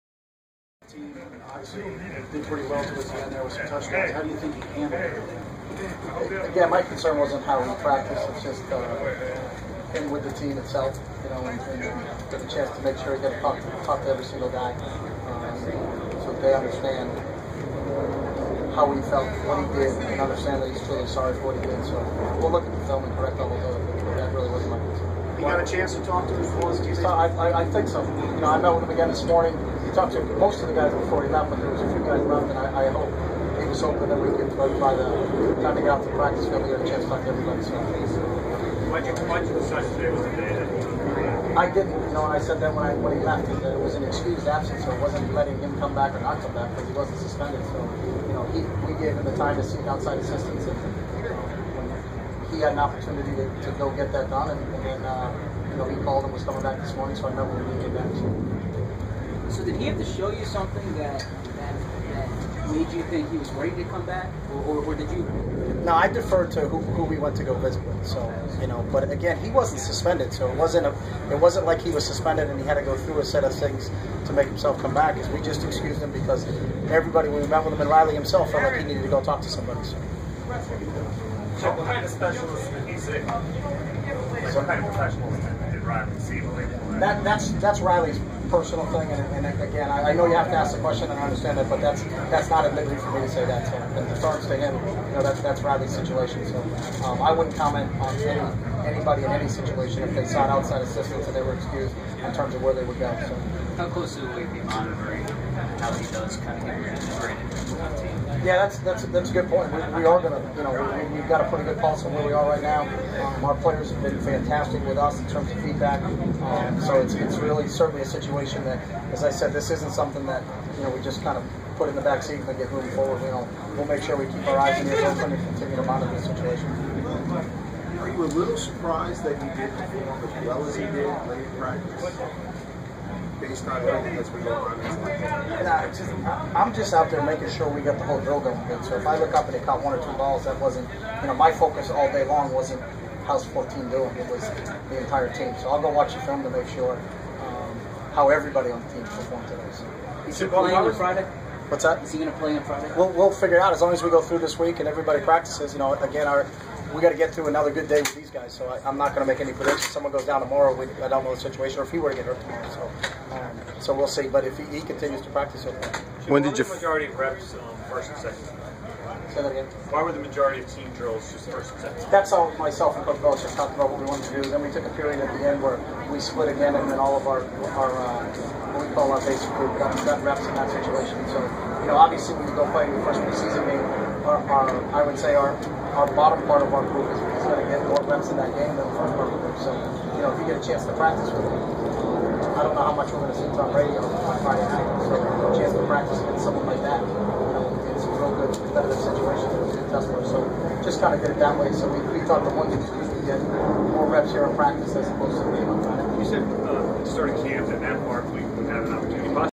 Eagles' Chip Kelly talks about Riley Cooper's return
Eagles coach Chip Kelly discusses Riley Cooper's return